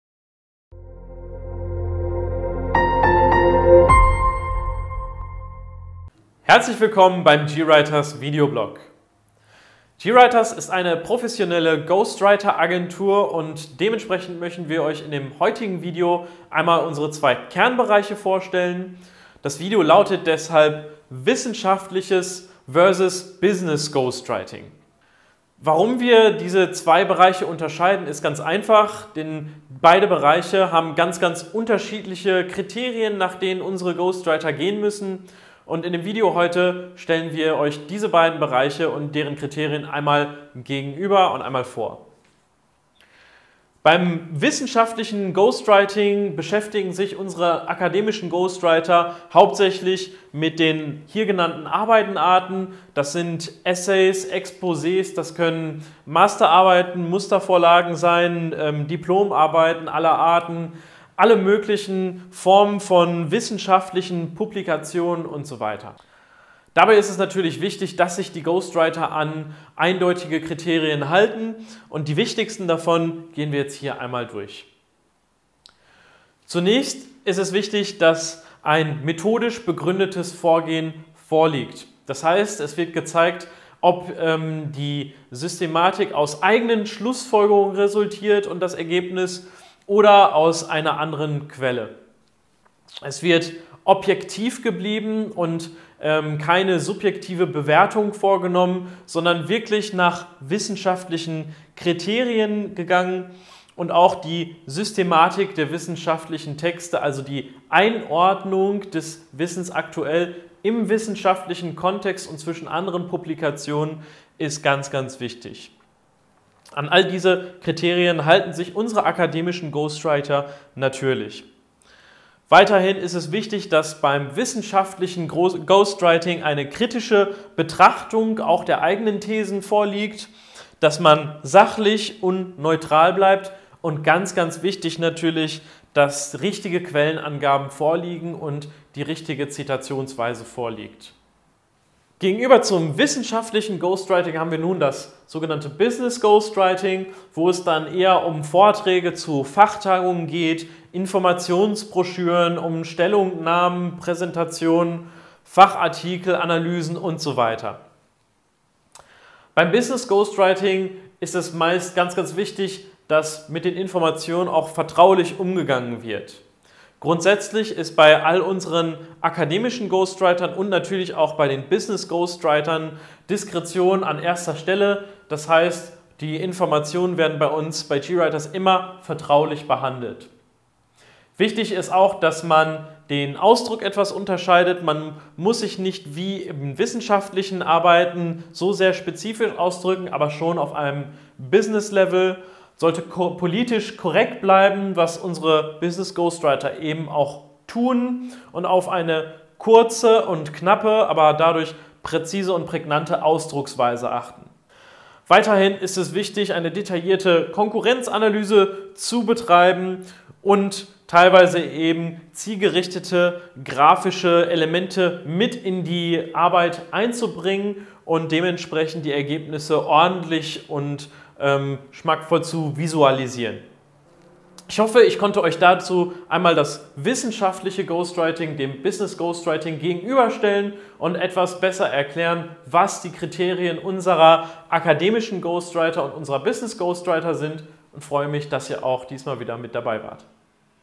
Wissenschaftliches Ghostwriting vs. Business Ghostwriting - Was steckt dahinter?
GWriters • • GWriters ist eine wissenschaftliche Agentur für akademisches Ghostwriting, Lektorate, Coachings und weitere Ghostwriting Services.

In diesem Video erklärt der Gründer Marcel Kopper, welche Unterschiede zwischen dem wissenschaftlichen Ghostwriting für Studenten sowie Forscher und dem Business Ghostwriting für Unternehmen bestehen. Detailliert und strukturiert listet er die Kerneigenschaften der zwei verschiedenen Ghostwriting-Services auf und stellt alle Besonderheiten gegenüber.